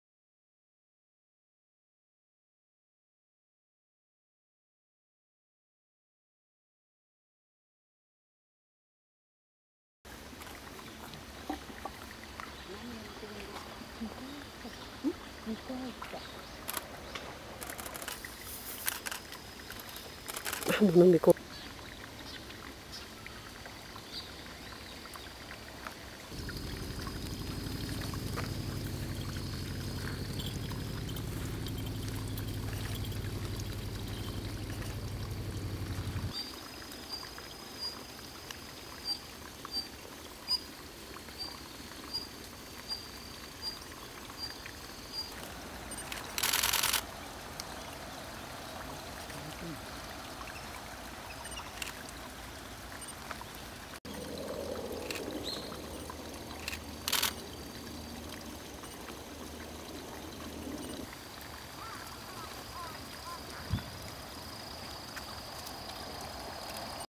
ミコアイサお譲さん～♪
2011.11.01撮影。
画面から飛び出すほどの近さで写せました。
雄も一緒だったらもっと良かったのですけどね。